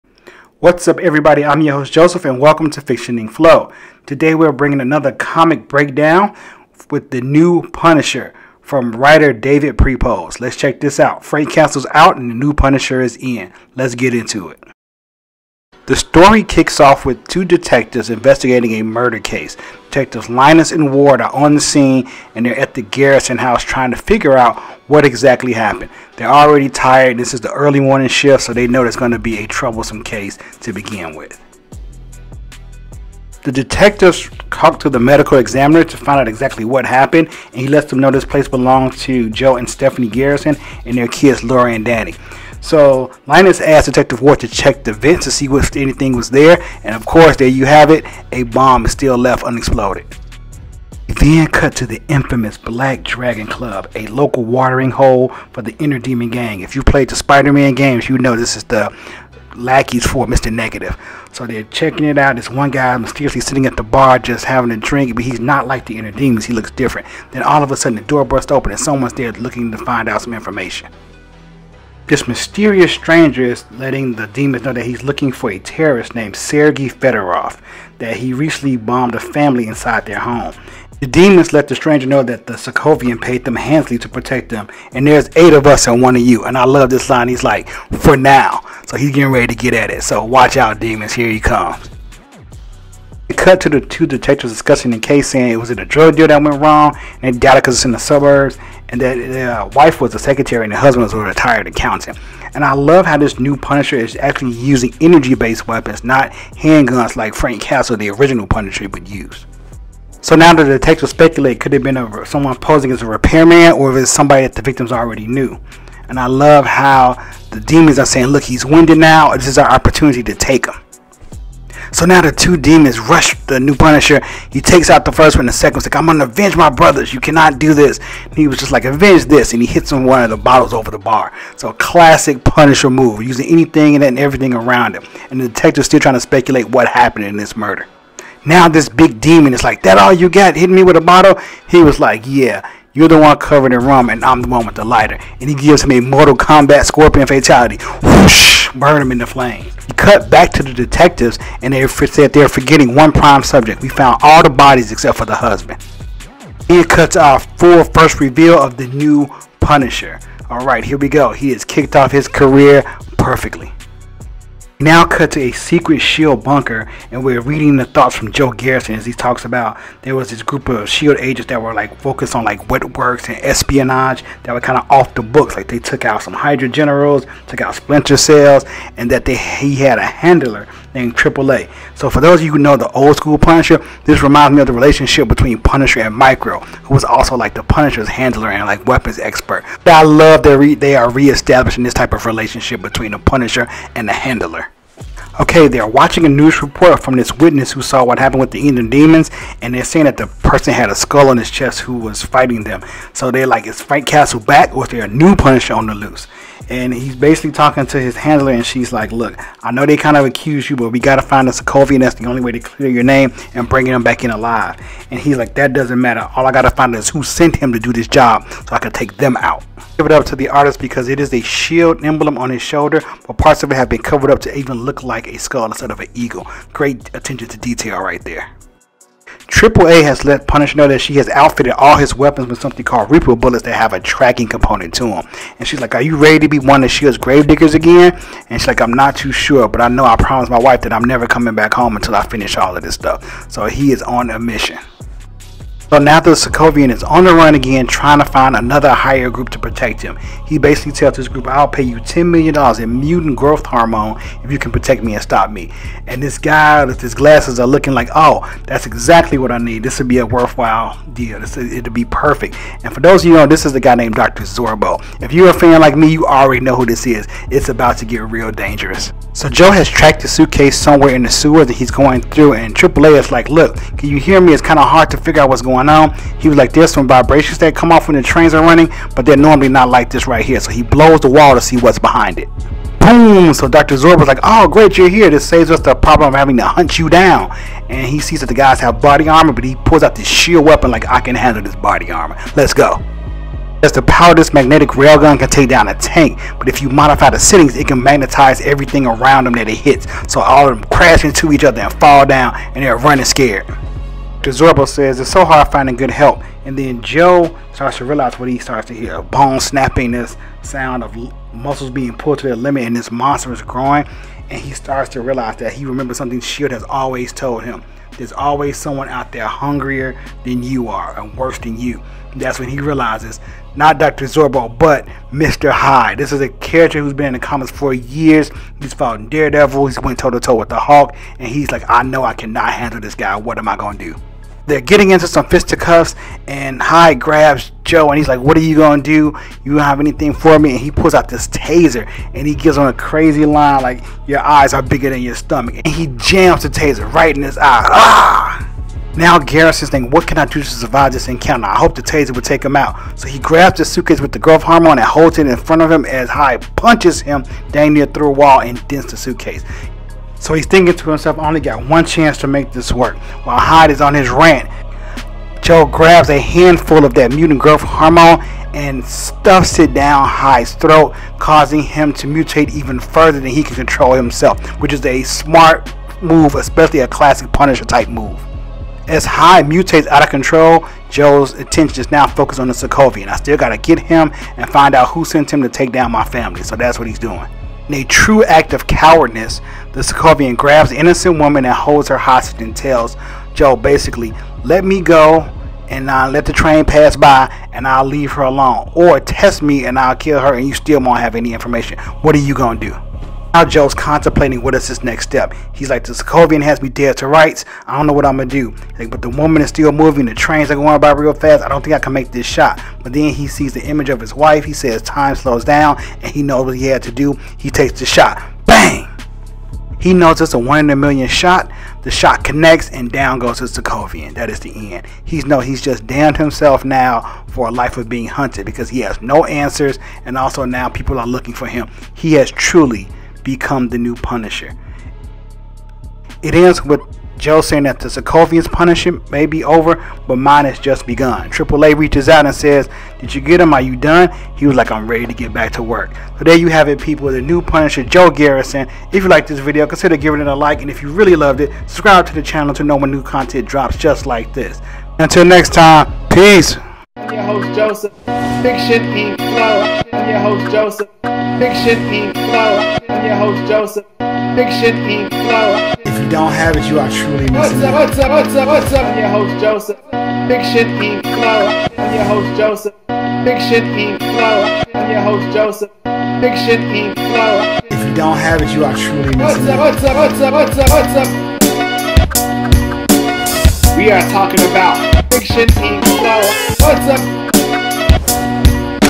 Who Is The New Punisher? Frank Has Competition!
There is a new Punisher in town and Frank Castle may have something to worry about. Can't wait for a meet up and battle between these two.